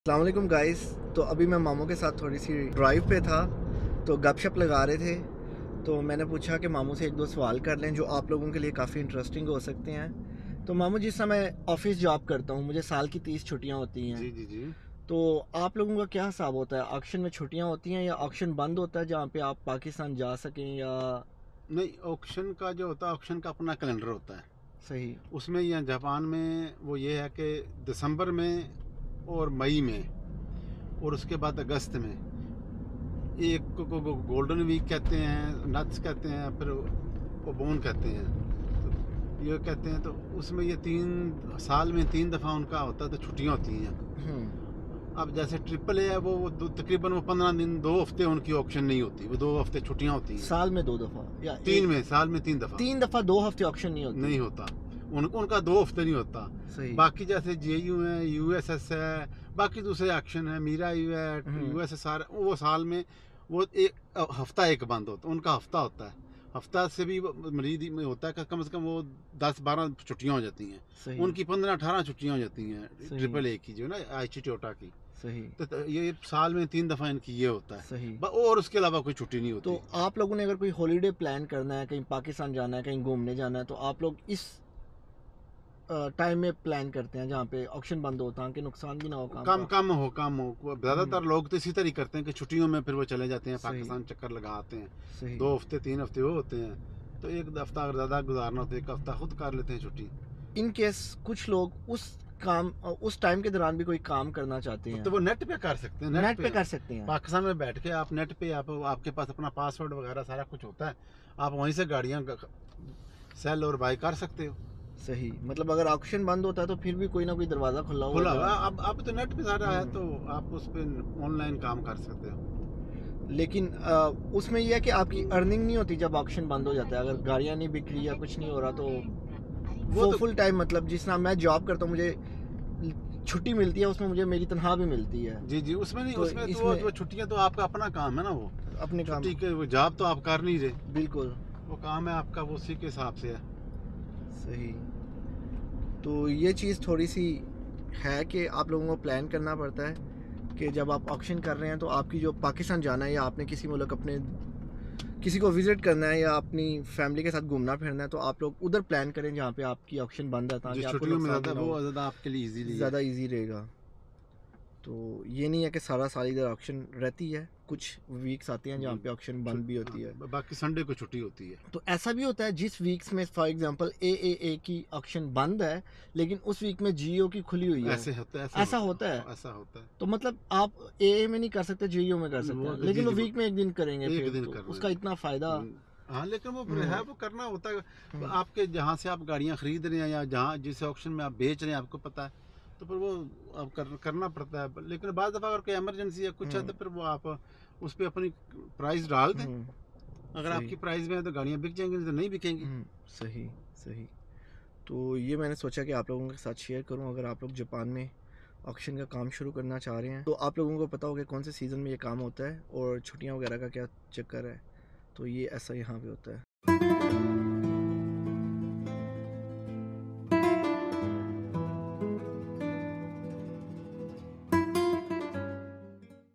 Can We Do Car Auction Business All Year in Japan?
Can We Do Car Business All Year in Japan?
In this video I will tell you about the car season in japan. My uncle will tell you about car auction business in Japan. Car auction business in Japan is very famous and many Pakistan is working on it.